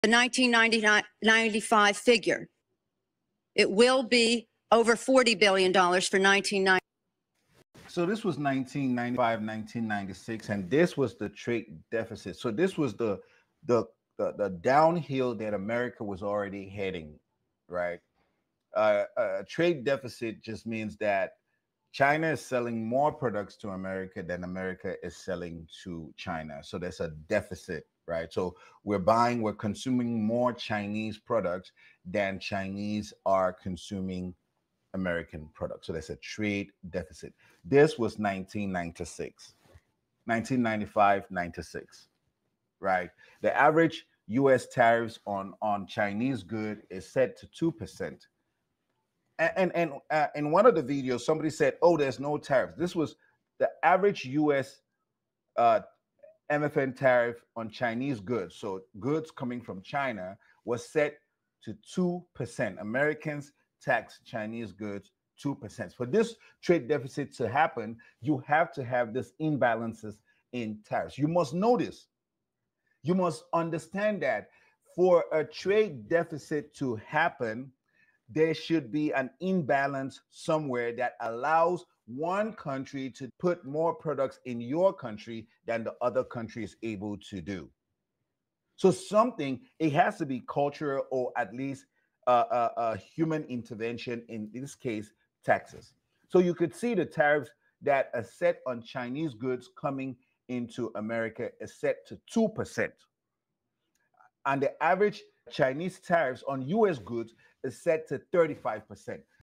Unpacking America's Trade Deficit: Insights from 1995
Discover the dynamics behind America's trade deficit with China, exploring tariff imbalances and economic implications. We break down statistics from the mid-90s that still resonate today, shedding light on consumer behavior and international trade.